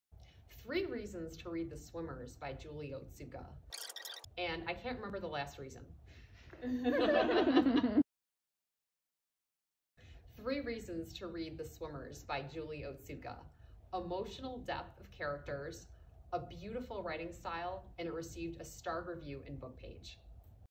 Super Short Recommendations: The Swimmers by Julie Otsuka
Fiction Librarian Allison would love to tell you three reasons to read The Swimmers by Julie Otsuka, but she forgot one of her reasons. Totally kidding! She remembers. Watch to find out why this is a wonderful read, then check it out